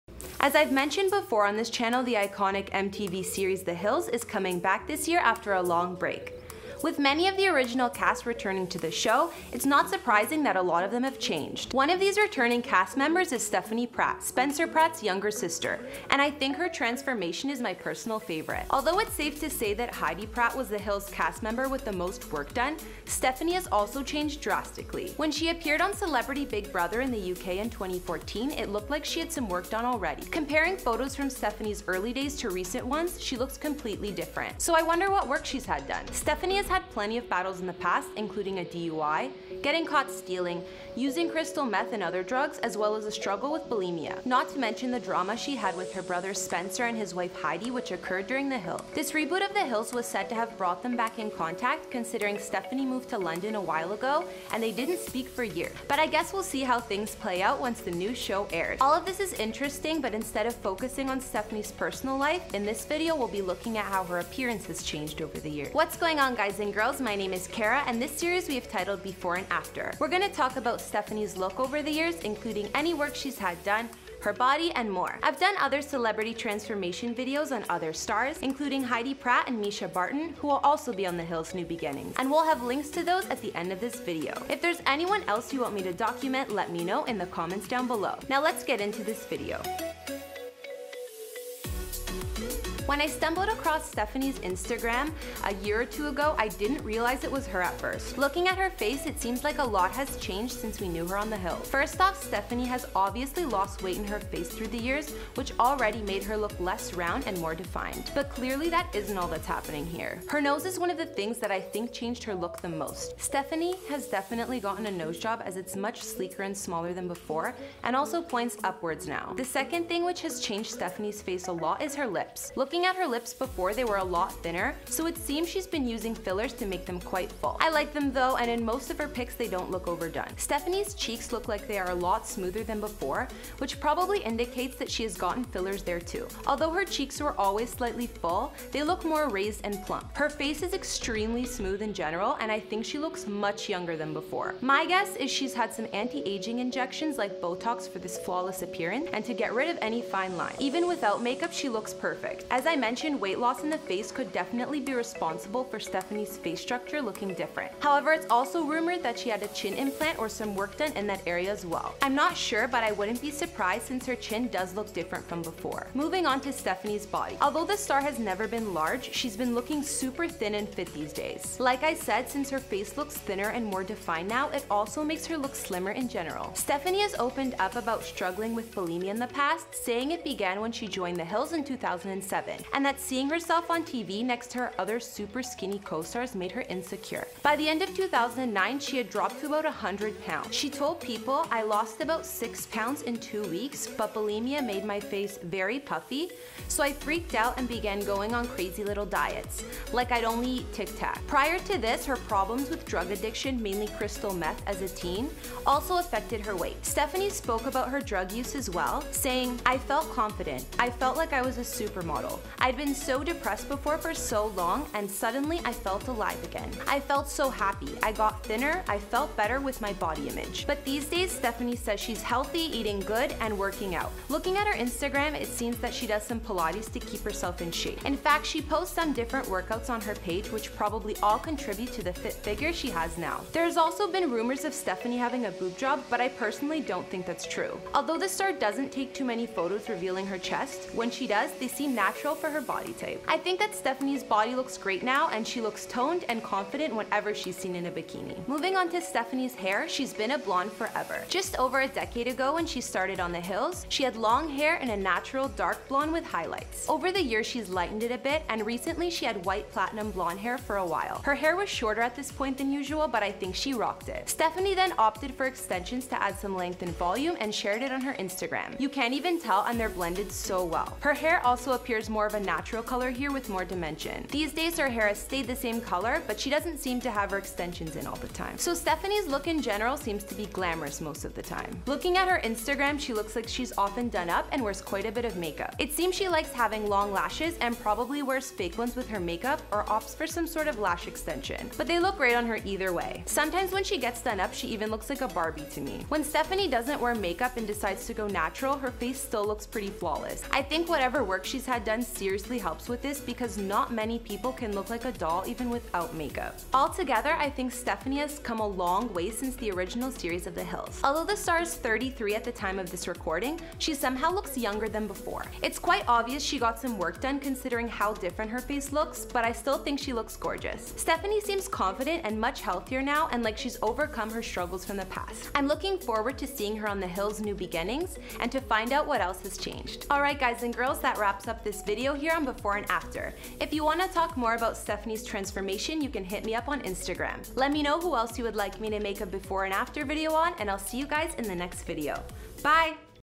Stephanie Pratt 2019 Glow Up | Before & After Transformations | The Hills: New Beginnings
Stephanie Pratt appeared on The Hills alongside her brother Spencer Pratt, Lauren Conrad, her sister in-law Heidi Montag Pratt, Audrina Patridge and Whitney Port. But has The Hills: New Beginnings star gotten any cosmetic surgeries done? Does she achieve her level of beauty with just makeup or has she gotten some work done?